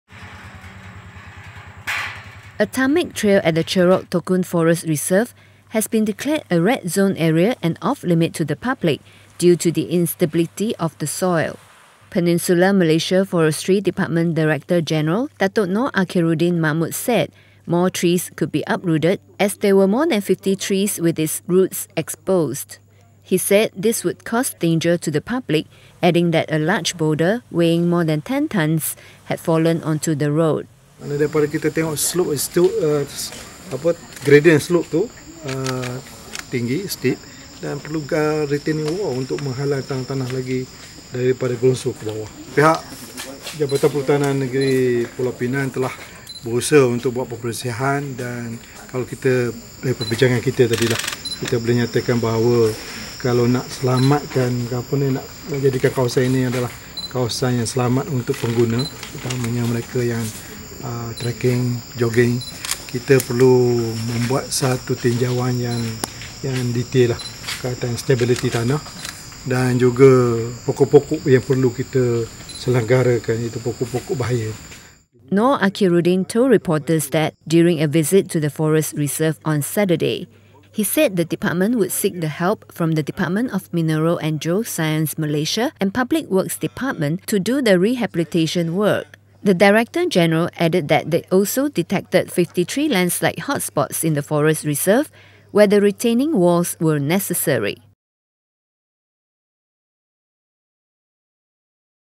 Taman Rimba Bukit Mertajam will be closed indefinitely
Peninsular Malaysia Forestry Department director-general Datuk Nor Akhirrudin Mahmud announced on Saturday that Taman Rimba Bukit Mertajam will be closed to the public until further notice. Nor Akhirrudin said this was due to the instability of the soil which could cause danger to the public.